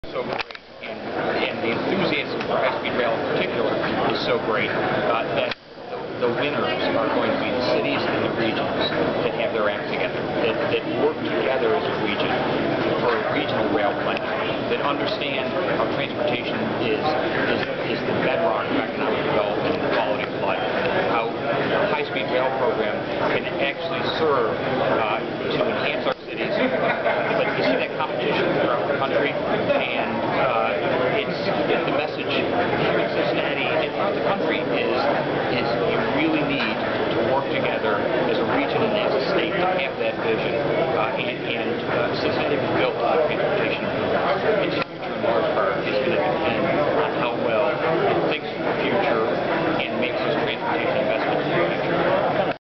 John D. Porcari speaks to high-speed rail event in Cincinnati on Wednesday
John C. Procari, Deputy Director for the U.S. Dept. of Transportation, speaks to a gathering of high-speed rail John D. Porcari, Deputy Director at the U.S. Dept. of Transportation, speaks to backers and vendors at a high-speed rail conference in Cincinnati on Wednesday May 5, 2010